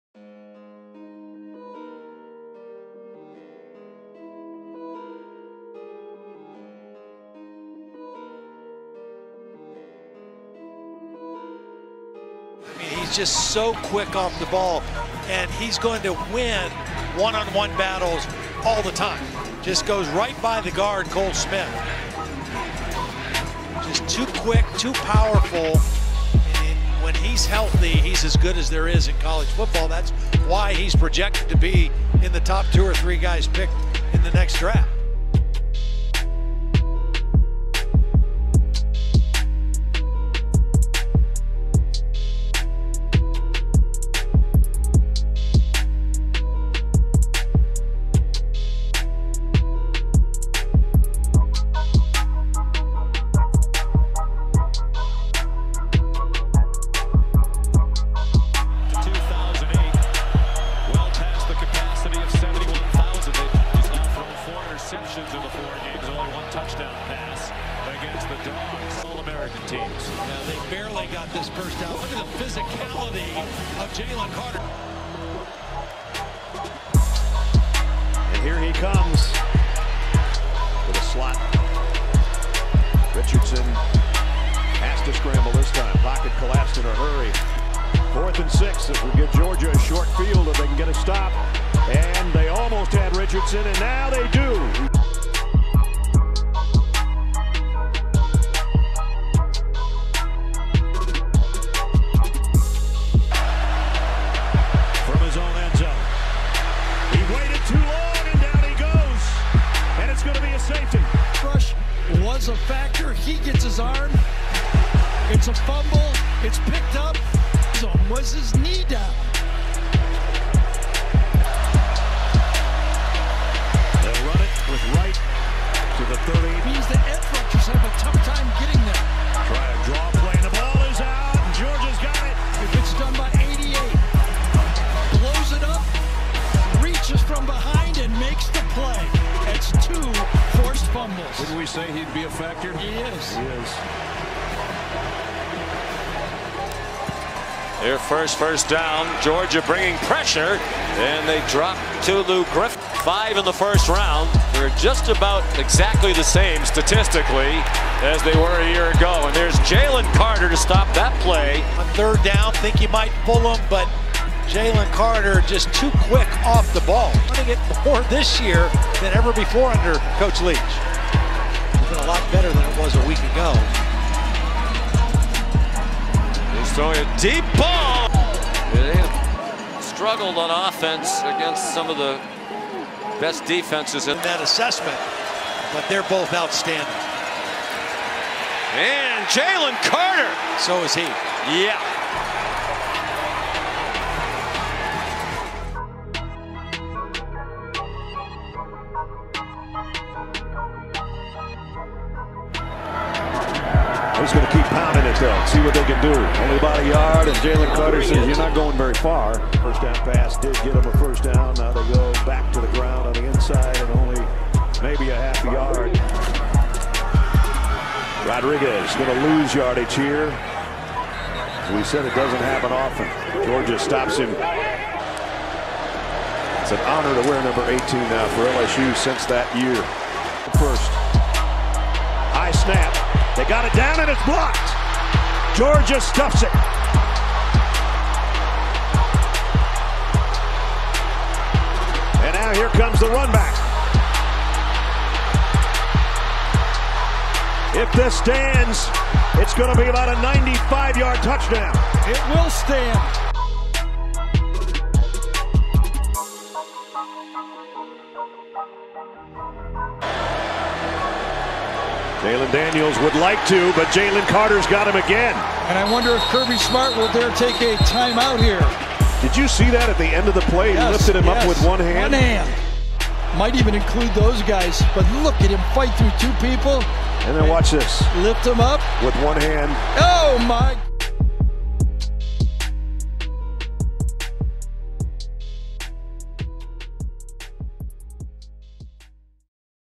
The Scariest DT in the 2023 NFL Draft 🔥 Jalen Carter Career Highlights ᴴᴰ
Check out Georgia's Jalen Carter Full Career Highlights! Combining elite athleticism with scary power he will be one of the first players drafted in the 2023 NFL Draft